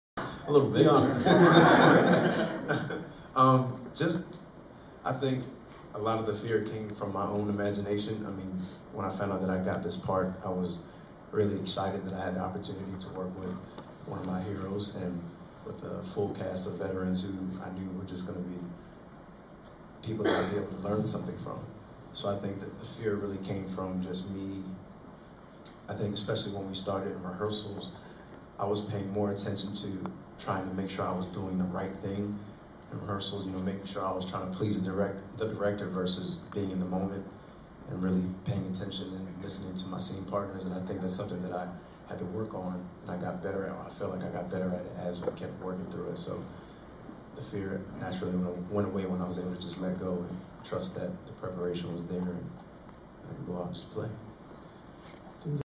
Jovan Adepo ('Fences') on feeling intimidated by Denzel Washington
Jovan Adepo ('Fences') explains his approach to playing a pivotal role alongside elite, veteran actors..

for the latest awards news and to make your predictions.